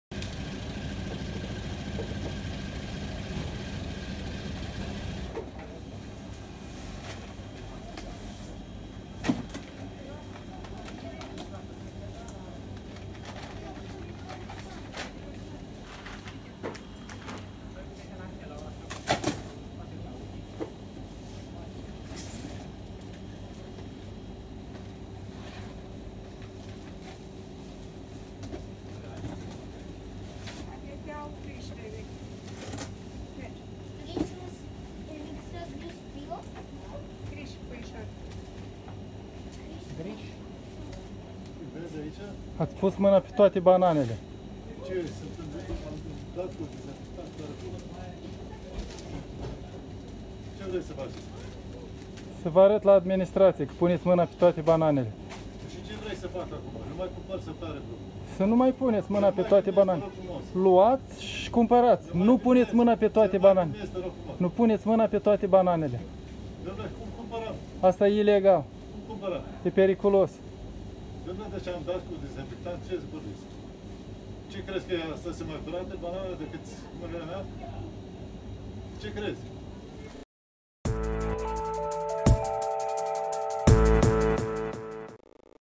A pus mîna pe toate bananele din 2 lăzi, a vrut și a 3-a - Curaj.TV
Un cumpărător exigent a achipuit cu mîinile fiecare banană din două lăzi începute deja la Lidl, Valea Oltului și avea chef să mai caute și în a treia.